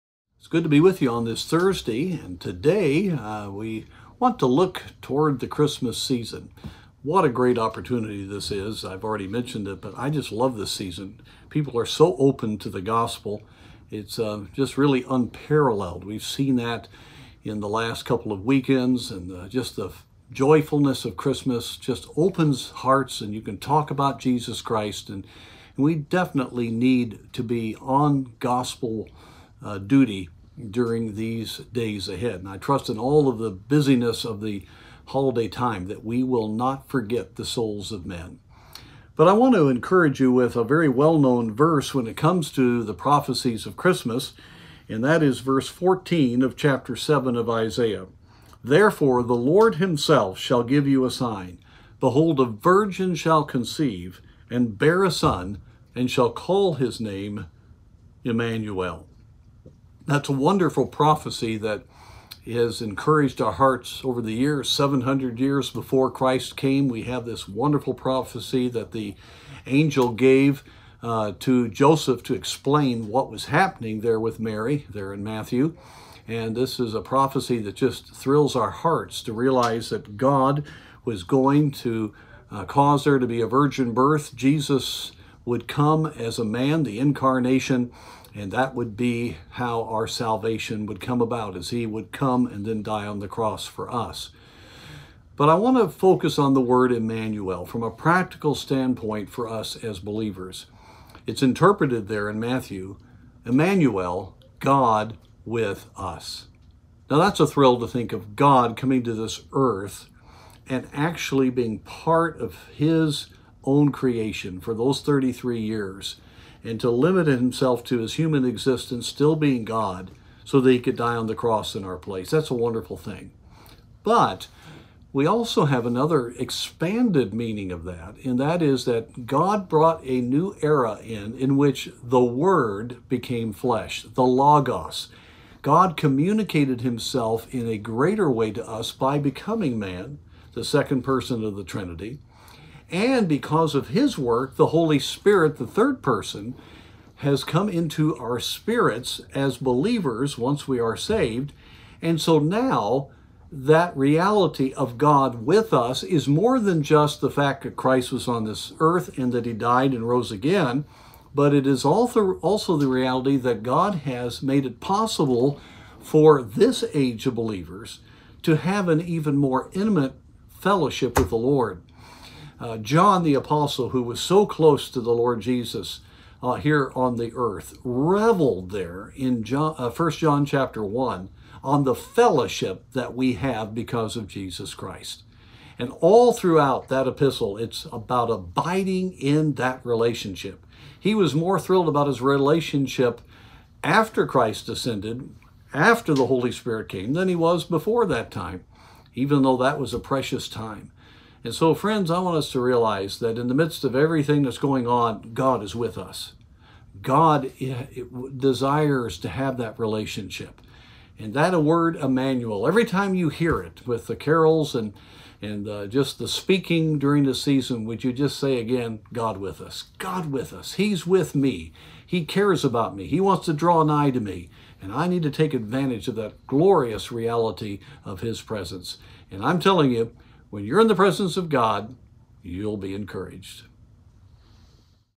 A Prophecy Prevails - Isaiah 7:14
Pastor Wayne Van Gelderen shares Biblical truth that will bring hope and comfort in these uncertain days. May we draw closer to God through this time and impact those around us for eternity.